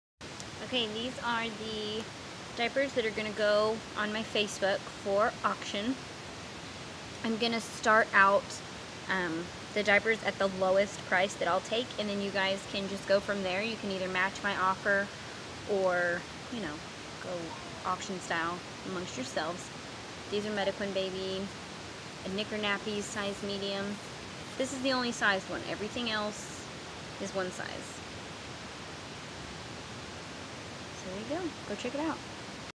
Diaper Auction! - Auction Over
these are the diapers that are in the diaper auction go check out my facebook!

Megan Rmmartin